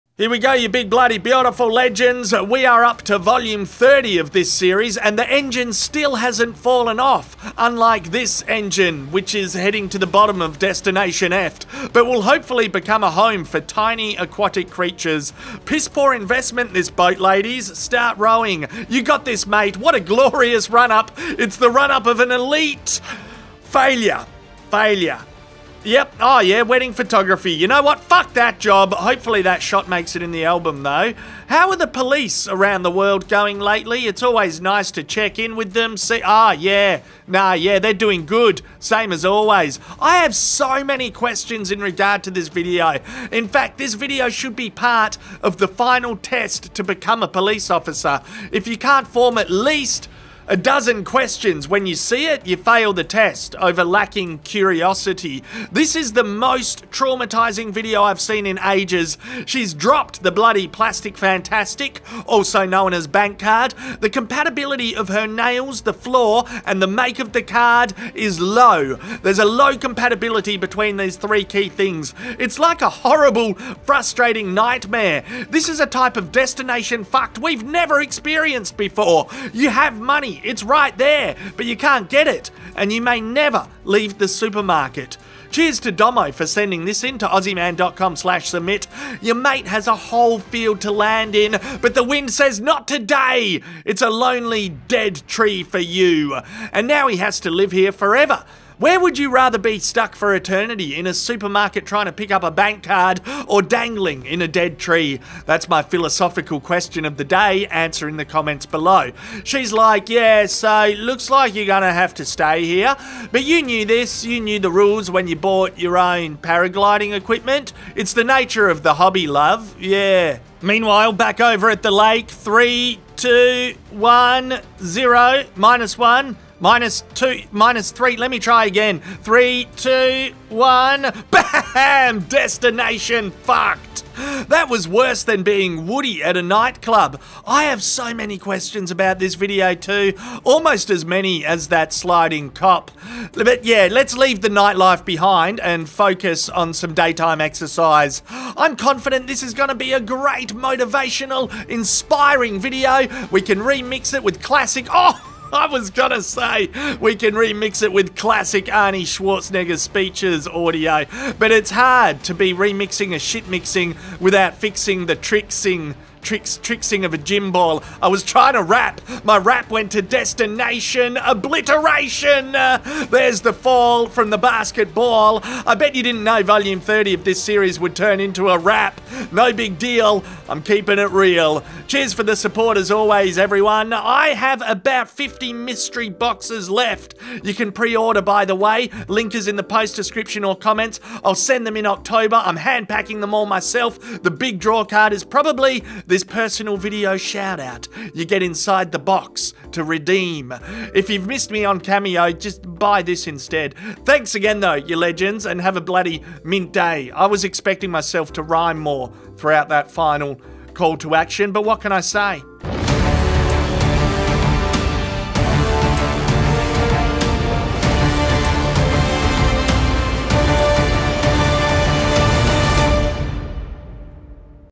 Destination Obliteration! - DF Compilation (Vol. 30)
Here's me commentary on Destination F'd Vol. 30 ya legends.

SAUCES:

Boat - Dunno
High Jump - Dunno
Wedding - Dunno
Police Slide - Boston Police
Plastic Fantastic - Dunno
Paragliding - Dommo in me inbox
Tubing - @moderndaymama on TT and IG
Woody - Nicola S.
Gym Ball - @jalessahelenolden on IG
Rebound - @cjprint24 on IG